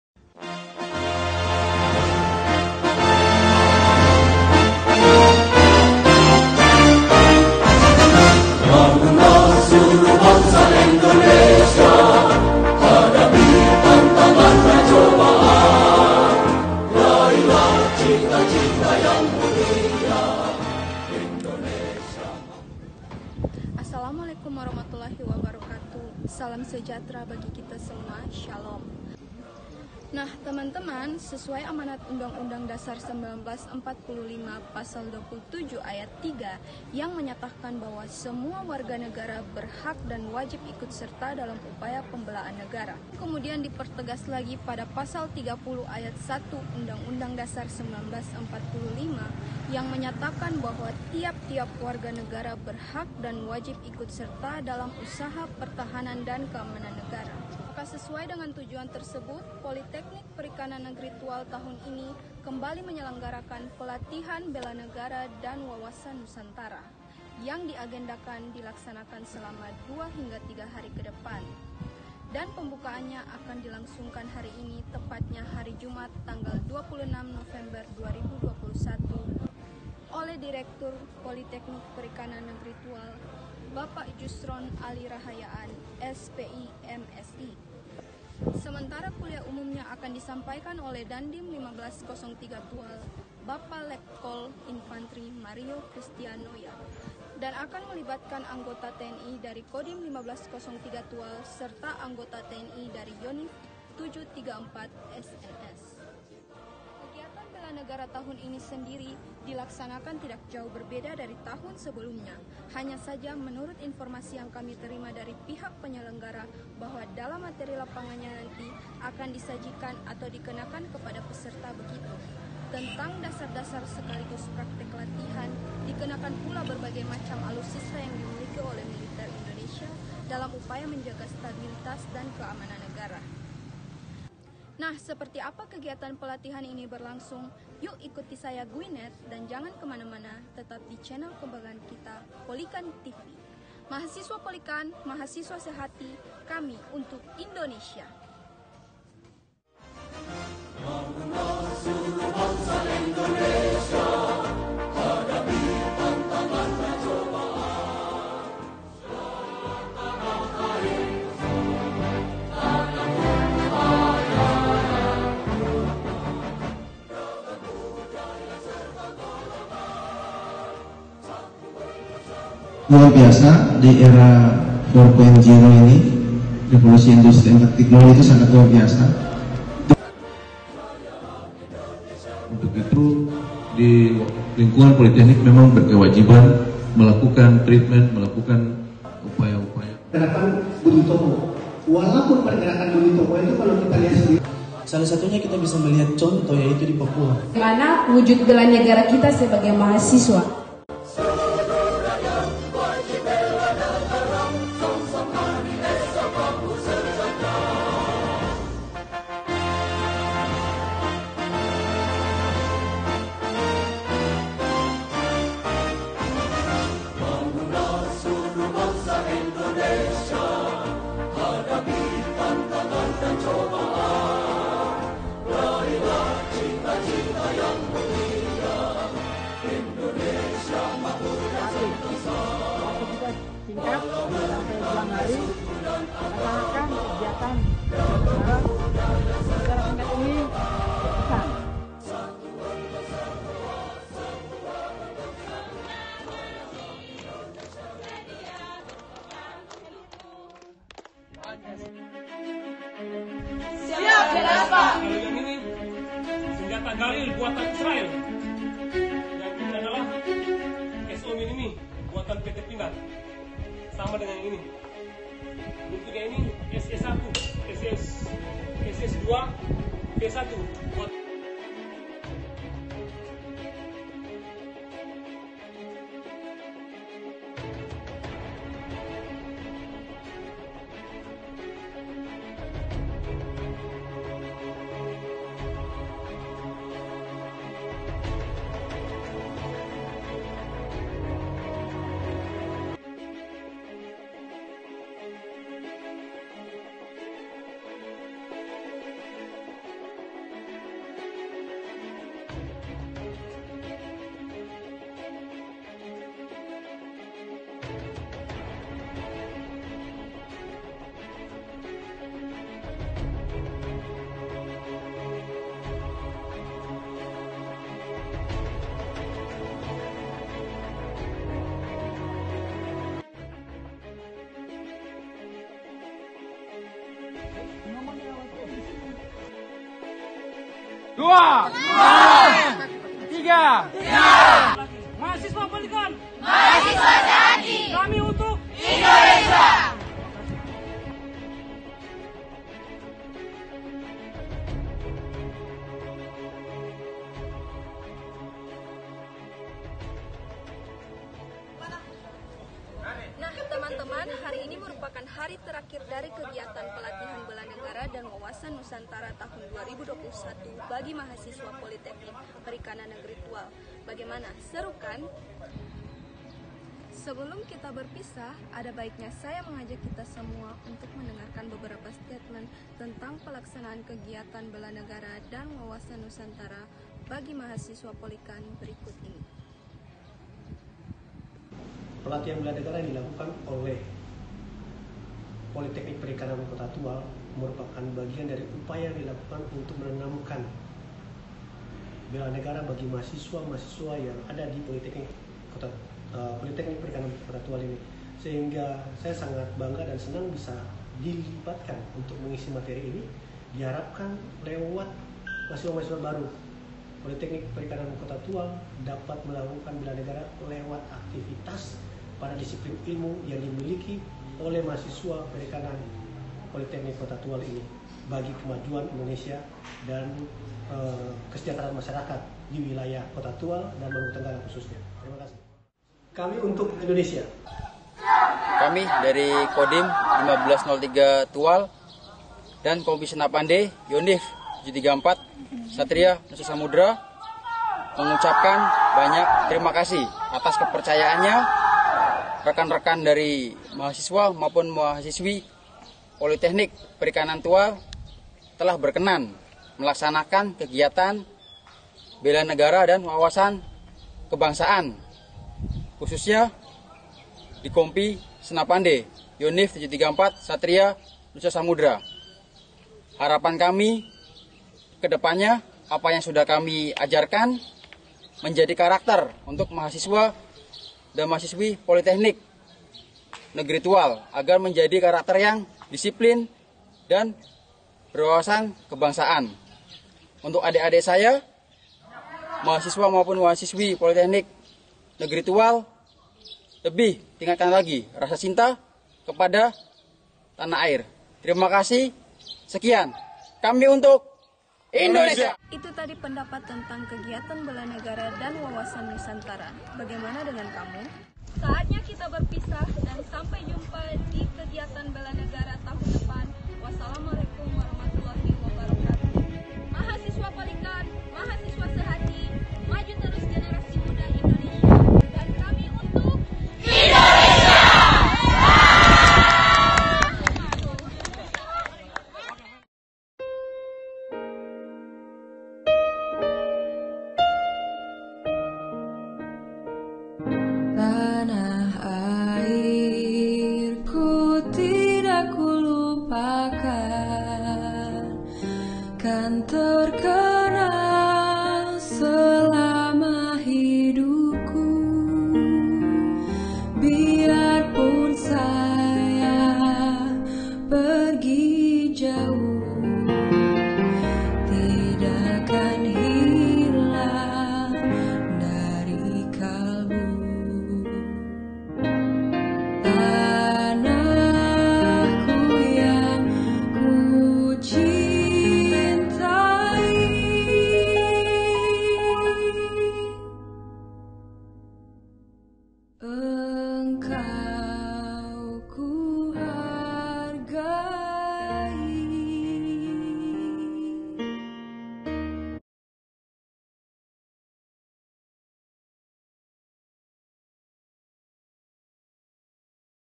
PELATIHAN BELA NEGARA BAGI MAHASISWA POLIKANT TAHUN 2021
Kegiatan Pelatihan Negara Negara dan Wawasan Nusantara bagi Mahasiswa Politeknik Perikanan Negeri Tual Tahun 2021.